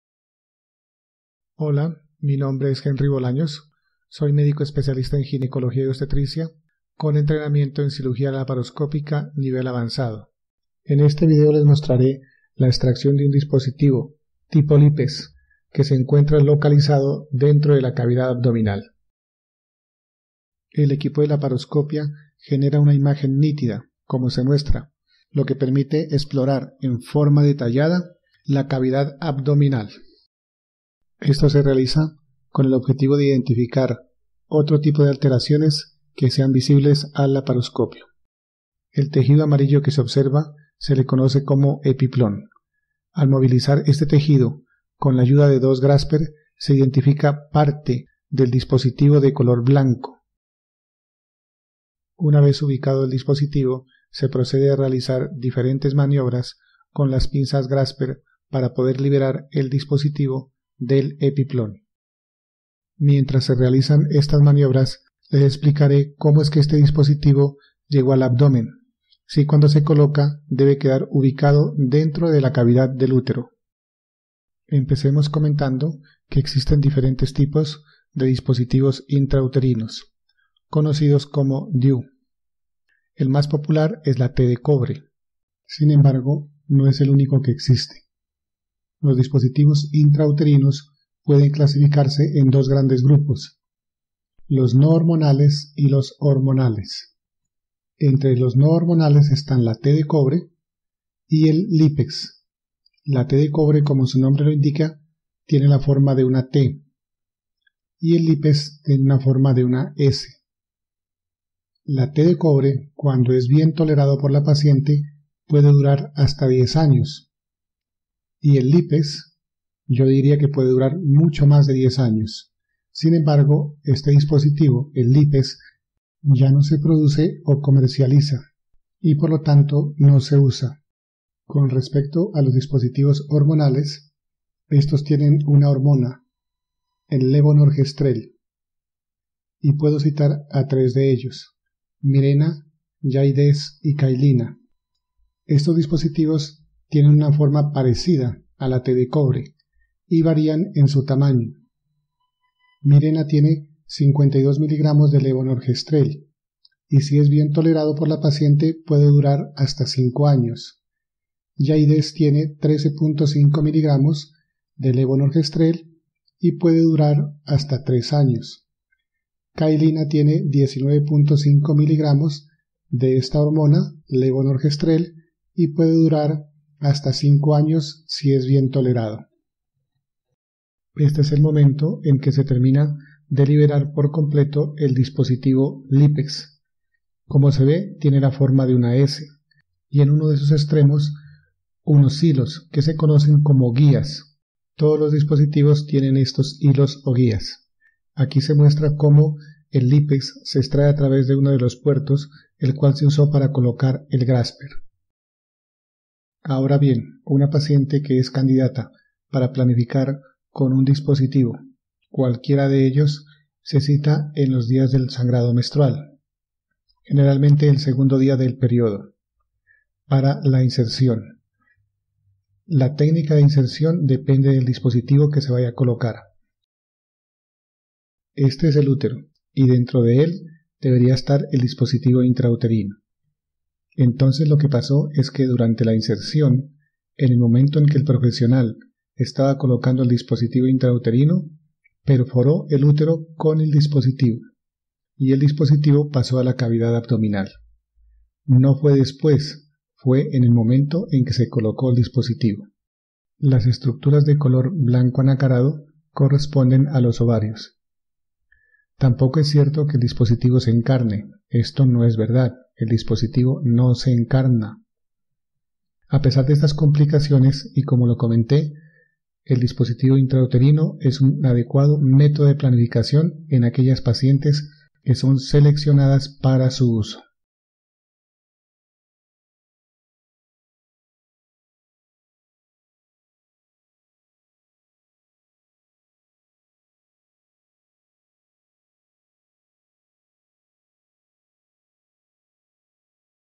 DIU Lippes intra abdominal
En este video, muestro la extracción de un DIU Lippes, localizado en la cavidad abdominal, además hablo sobre los diferentes tipos de dispostivos intrauterinos o DIU. Este video se sube con fines didácticos, para mejorar el aprendizaje de los profesionales de la salud